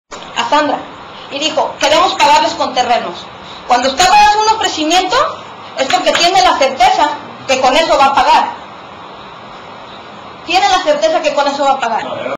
Cihuatlán sesión Ayuntamiento 29 Jul 2009 02
02. Resulta que la propuesta de pagar con terrenos no fue consultada por el Alcalde con los Regidores; cuando le dicen al Presidente que tiene doble discurso, el síndico Arturo Salas pide respeto...